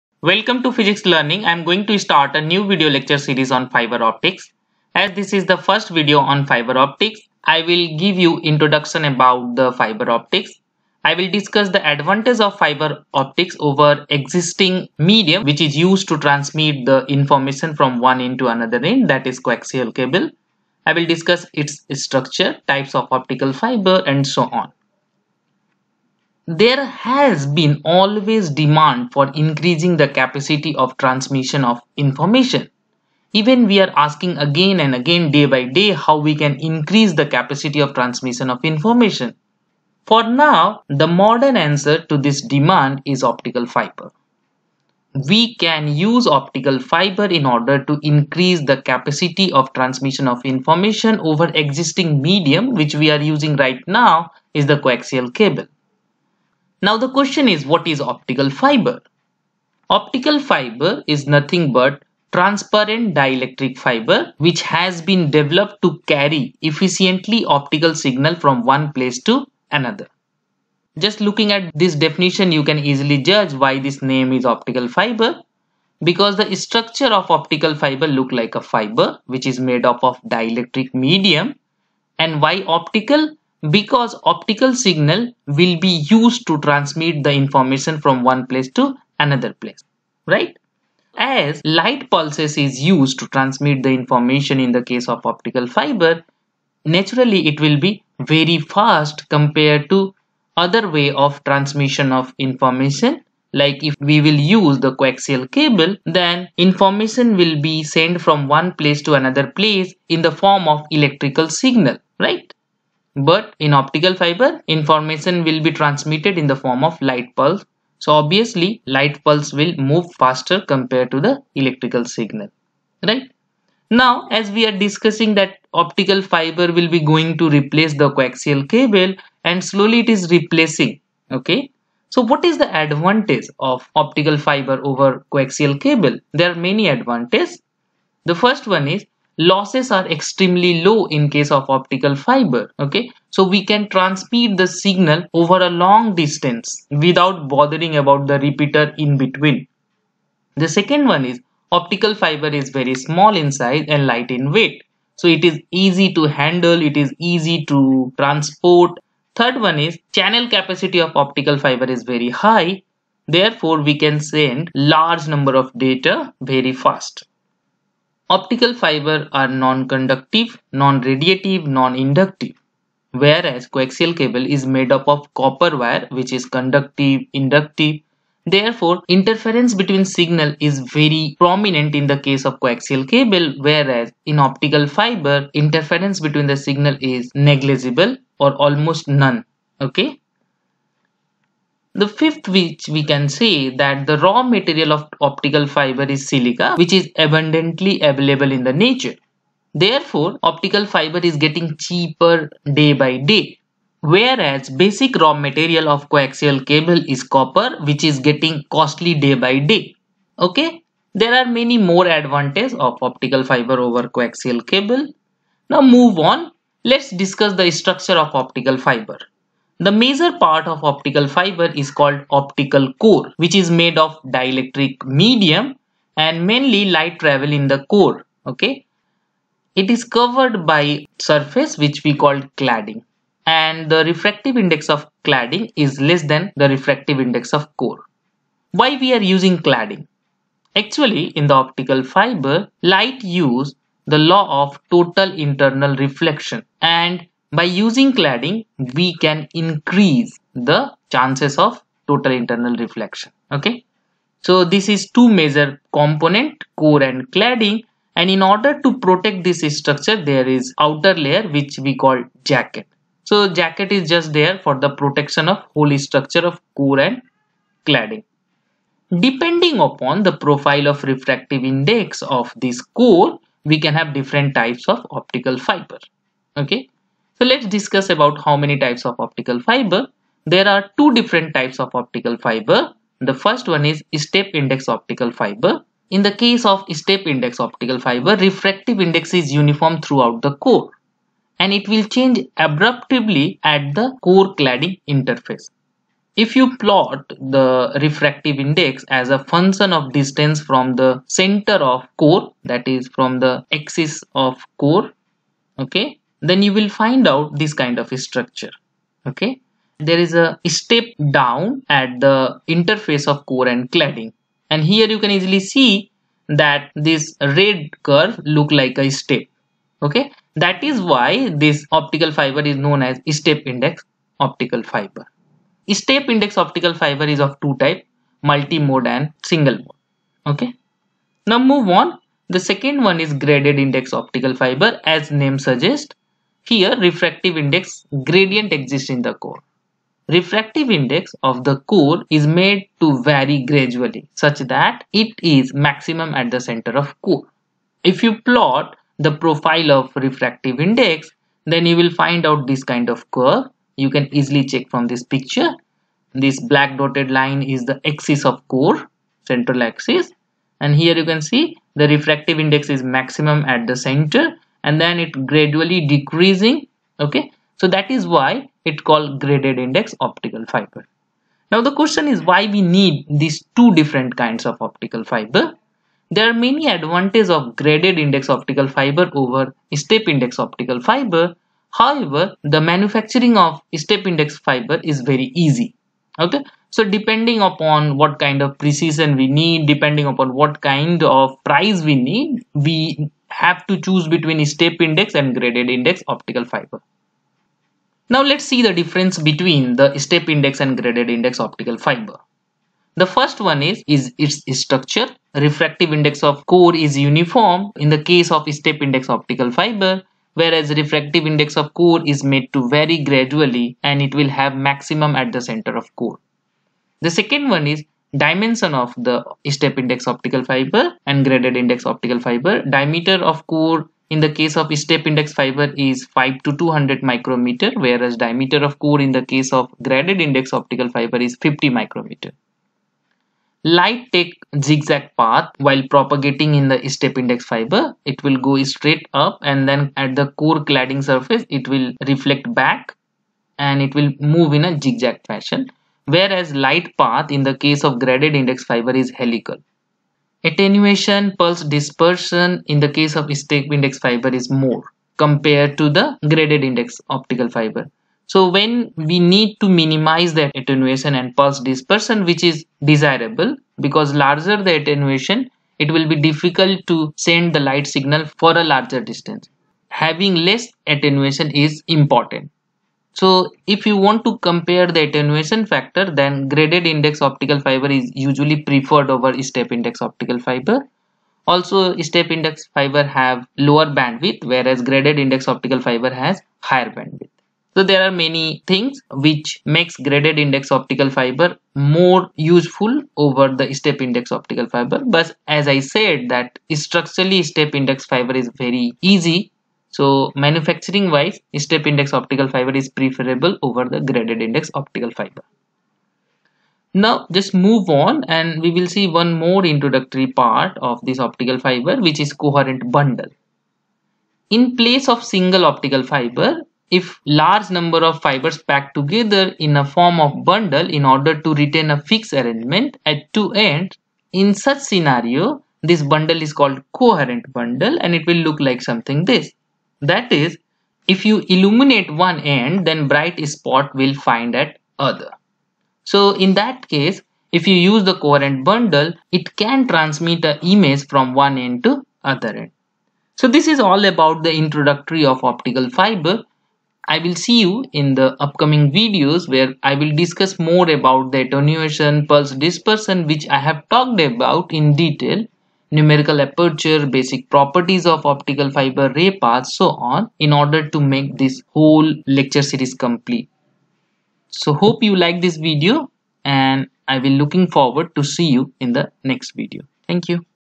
Introduction to Optical Fiber | Structure | Types of optical fiber | step index and graded index
In this video, I have discussed the following topics.

1. Introduction to Optical fibers
2. Structure of optical fibers
3. Advantage of optical fiber above coaxial cable
4. Types of optical fibers
5.Step index optical fiber
6.Graded index optical fiber
7.Difference between step index and graded-index optical fiber
8.Coherent bundle

Here, I have explained two types of optical fiber
1. Step index optical fiber
2. Graded index optical fiber

This video is part of a video series: FiberOptics. You are watching this video on PhysicsLerningWithDrShaw